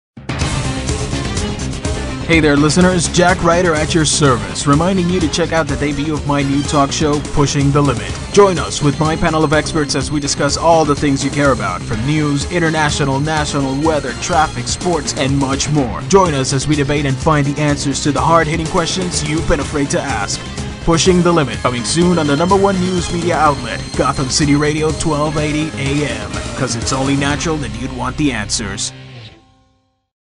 Gotham City Radio Promo 1 - Pushing the Limit
Coming soon an all new web audio series. As a tribute to all the creative forces at DC Comics. Stay Geeky my friends!